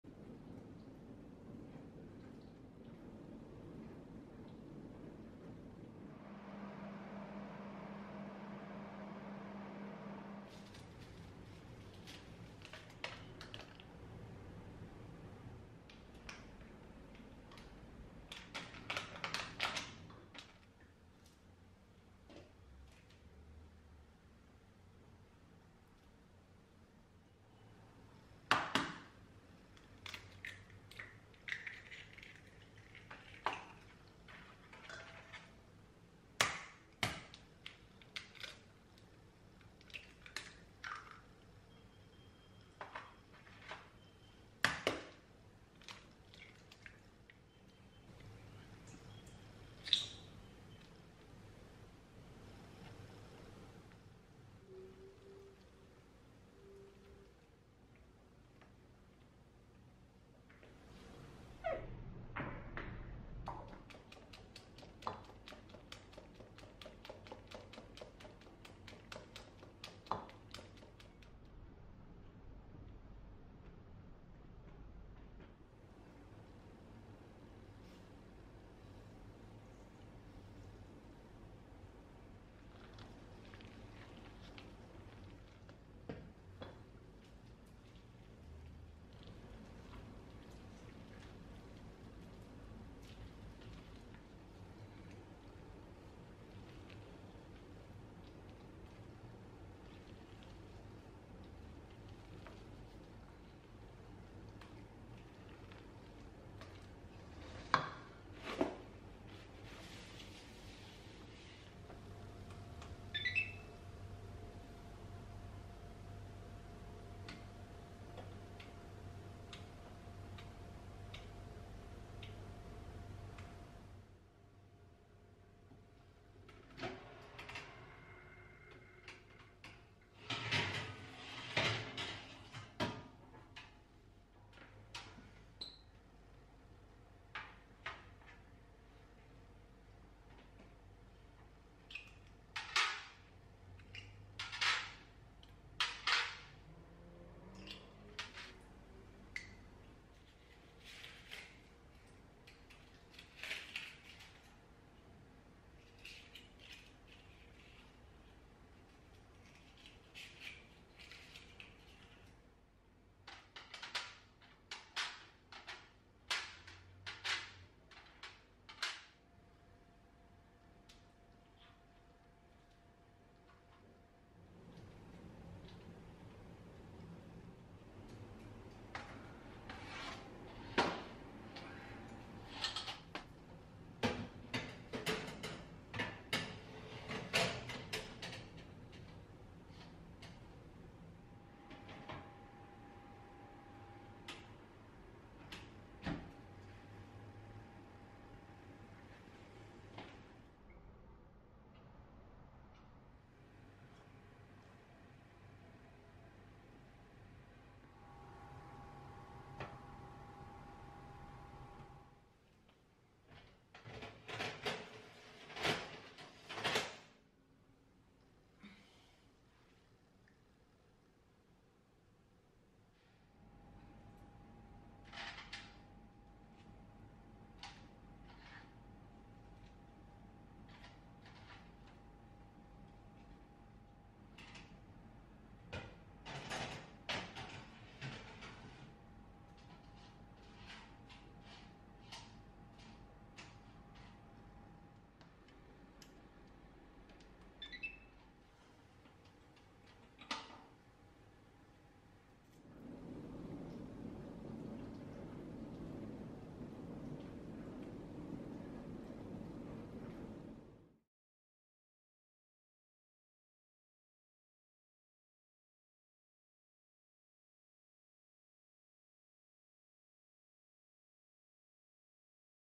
How to make coconut macaroons with 7 ingredients simple and easy recipe by kaye Torres
COCONUT MACAROONS ingredients

BROWN SUGAR      2 Spoon
Coconut shredded    2 cups
BUTTER unsalted     200gm
Baking powder   1 spoon
Condense milk       1 canned
Vanilla                      1 spoon

baked for 25 to 30 mins 160 degree c